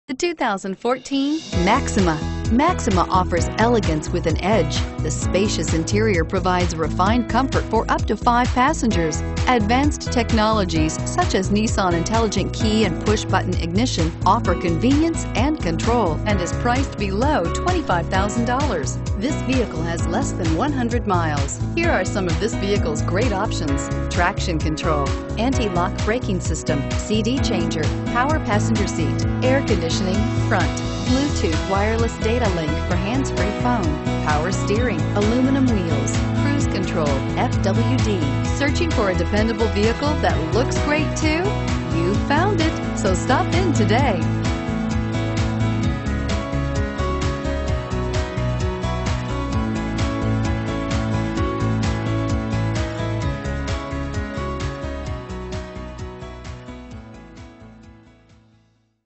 2014 Nissan Maxima Indianapolis IN C4339
2014 Nissan Maxima 3.5 S

You'll love this 2014 Nissan Maxima. This is a car you'll want to take home. With 7 miles, it features Automatic transmission and an exterior color of Super Black. Call and get in touch with Andy Mohr Nissan directly, and be the first to open the car door today!